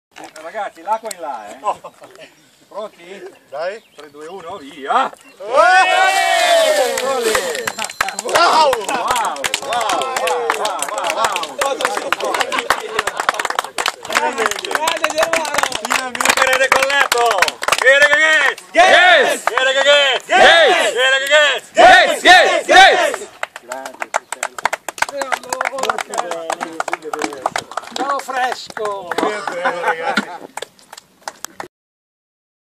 Doccia dopo il primo decollo da solista!
Primo decollo da solo pilota a bordo dell'aliante del nostro amico Germano!
L'immancabile doccia dopo l'atterraggio!